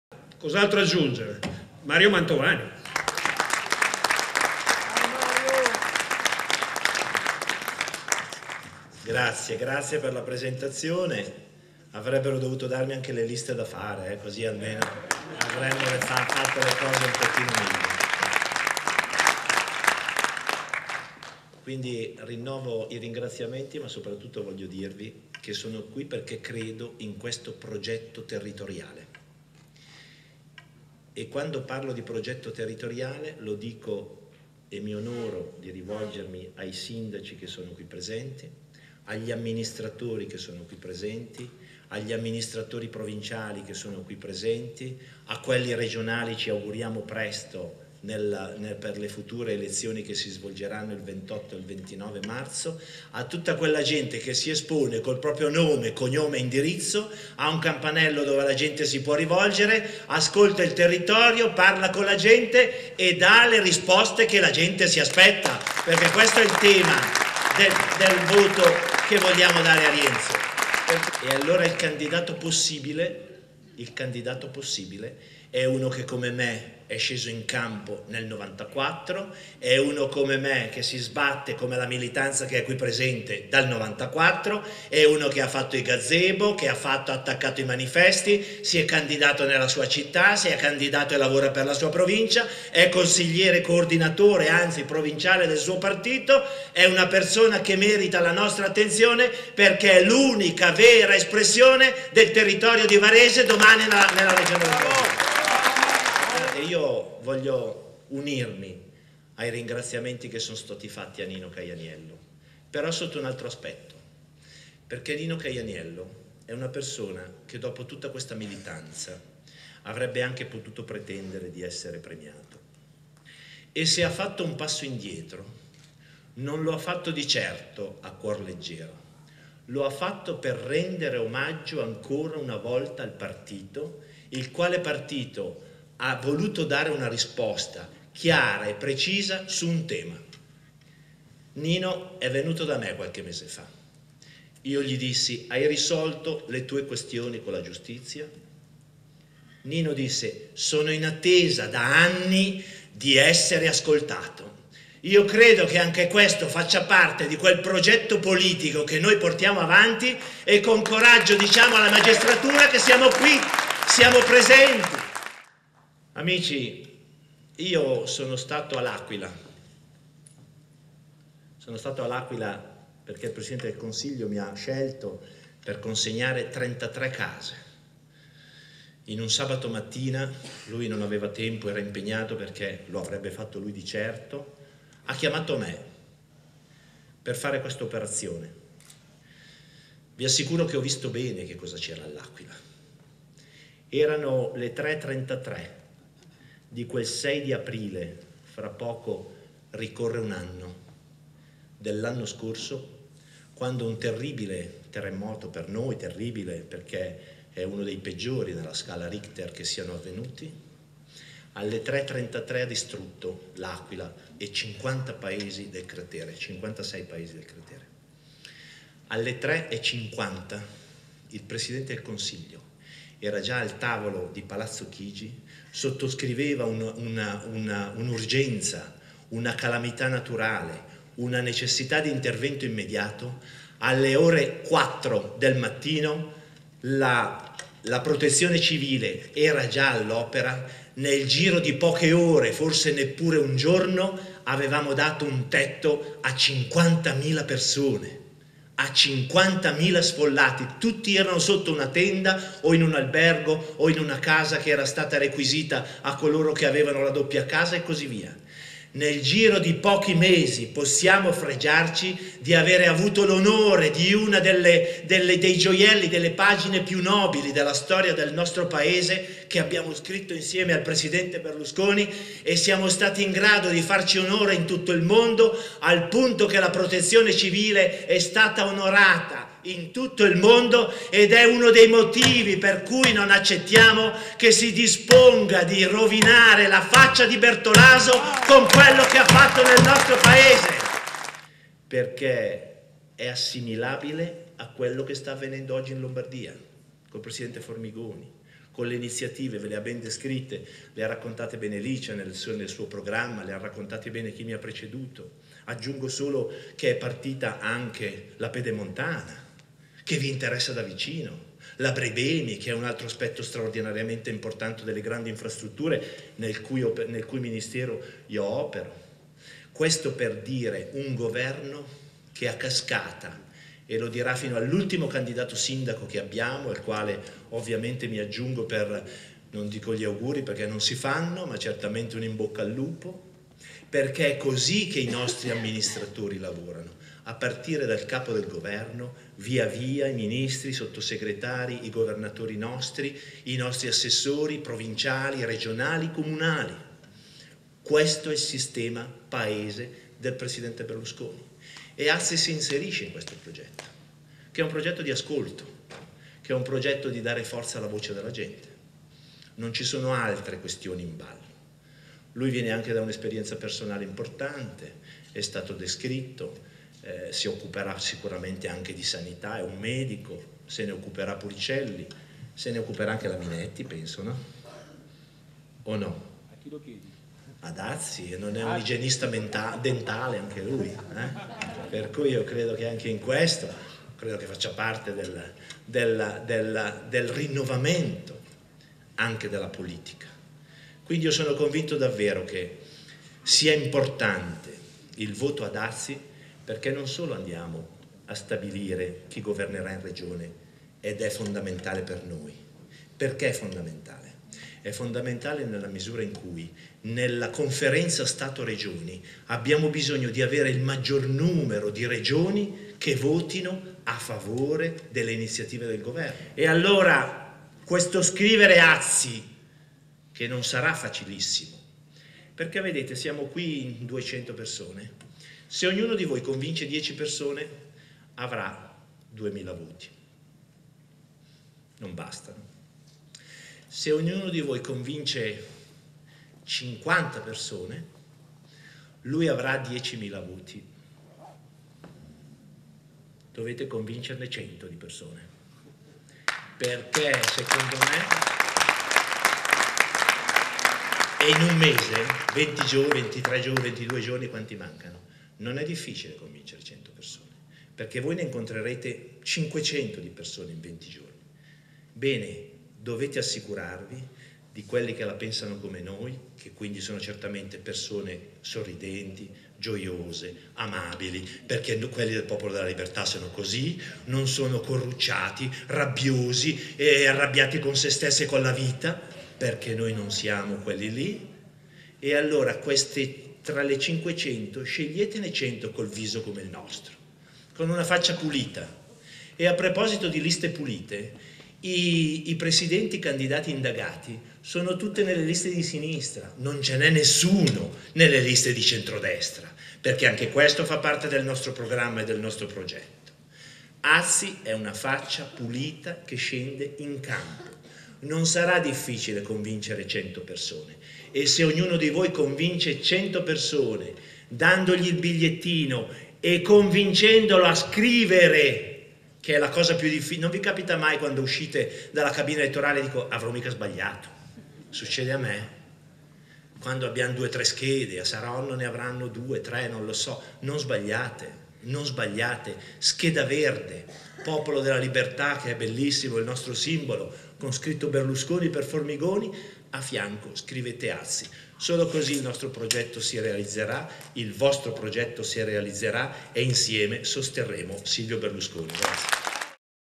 Mario Mantovani Per Rienzo Azzi
Presentazione di Rienzo Azzi, candidato per il Popolo della Libertà alle elezioni regionali del 28 e 29 Marzo 2010, presso il Teatro Politeama di Varese.
Sono intervenuti numerosi personaggi di spicco del mondo politico regionale e nazionale come:
Mario Mantovani, Lara Comi, Licia Ronzulli, Attilio Fontana, Luca Marsico, Nino Caianiello, Giorgio Puricelli ...